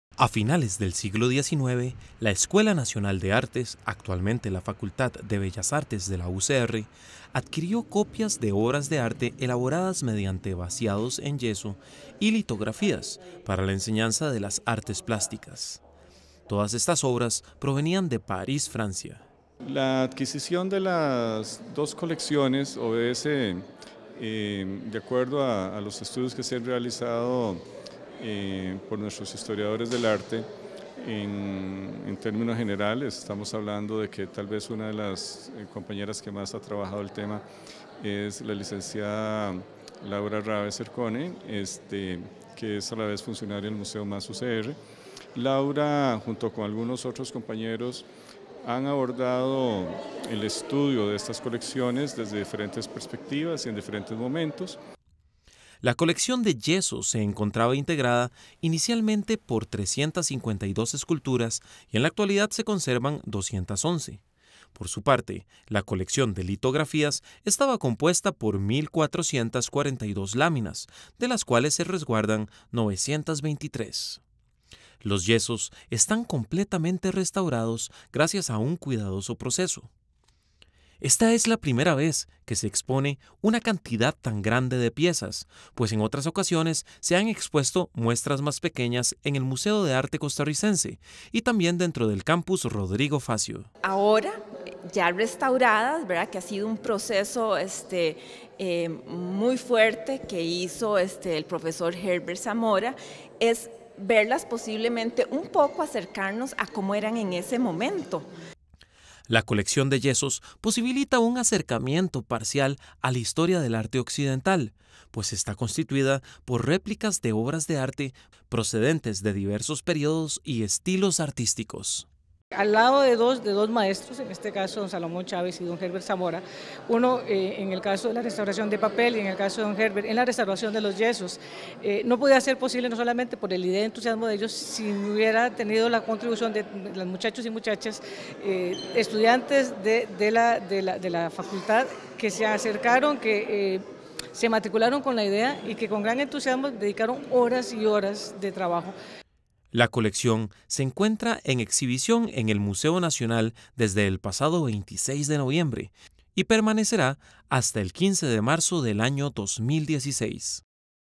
De París a San José: La colección de esculturas más antigua de la Universidad de Costa Rica
"De París a San José: La colección de esculturas más antigua de la Universidad de Costa Rica" es el nombre de la exposición que el Museo +UCR y la Escuela de Artes Plásticas de la UCR exhiben desde el 26 de noviembre en las instalaciones del Museo Nacional. Esta colección es una selección de piezas que datan del año 1897.